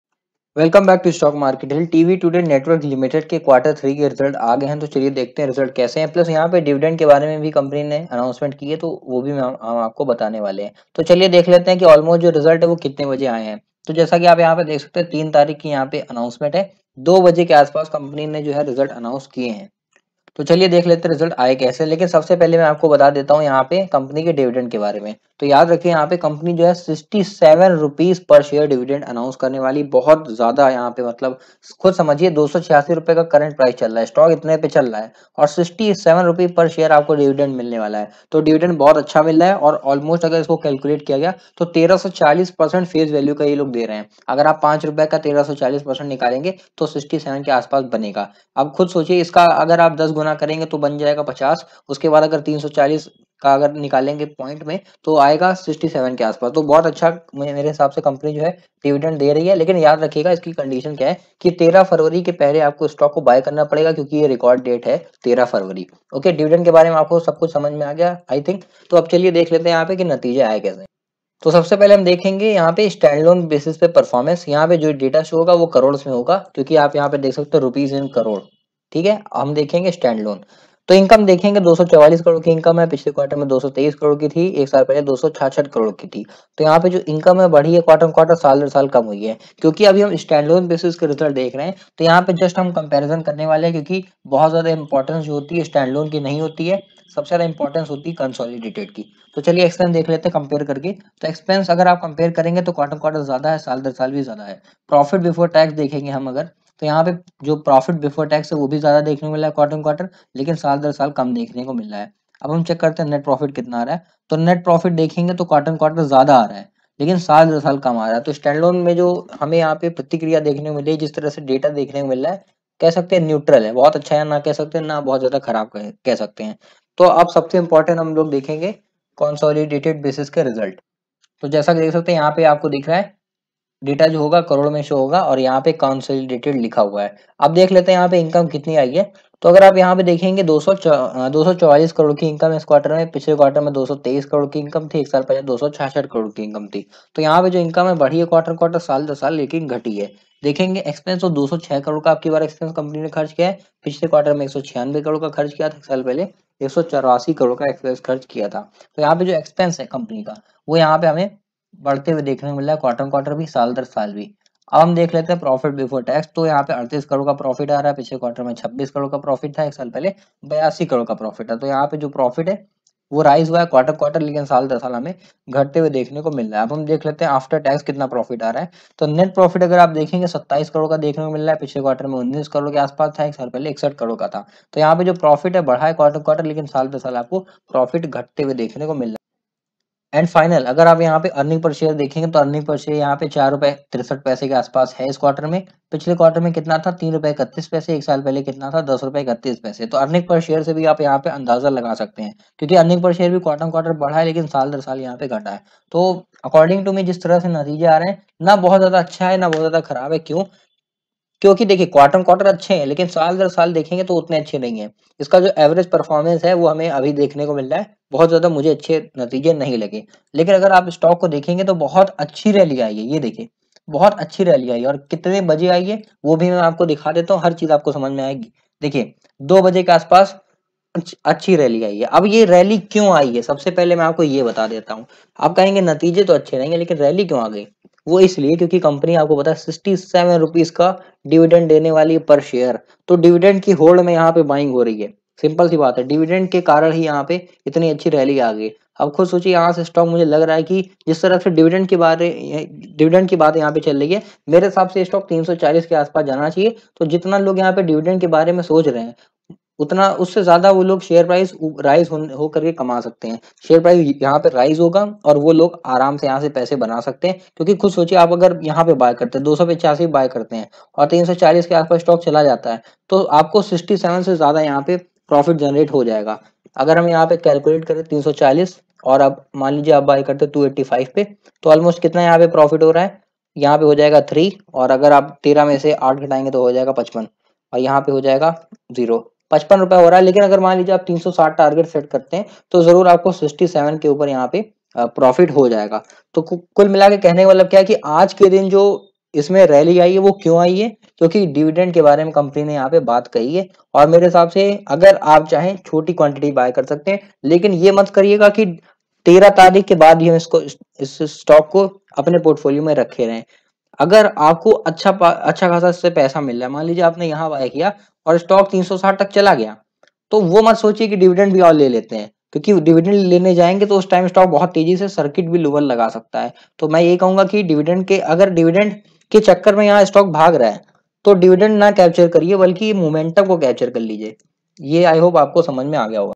TV Today Q3 Result || TV Today share latest news || TV Today Dividend
In this video I Have done fundamental analysis as well as technical analysis on TV Today.